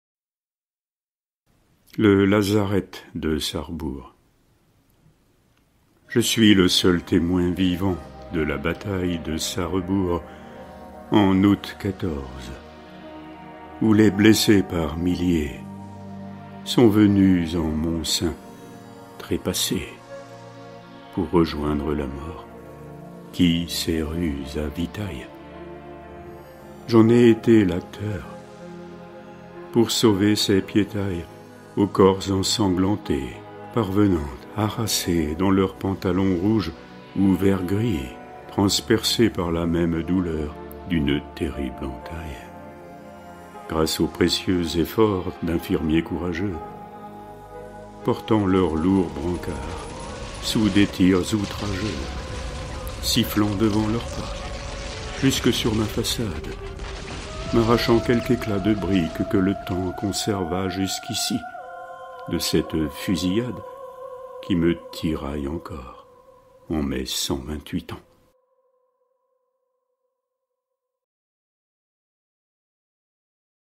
le lazarett de Sarrebourg
ancien hôpital militaire de Sarrebourg, témoin et acteur de la bataille de Sarrebourg du 18 au 20 août 1914 et hôpital arrière allemand durant toute la guerre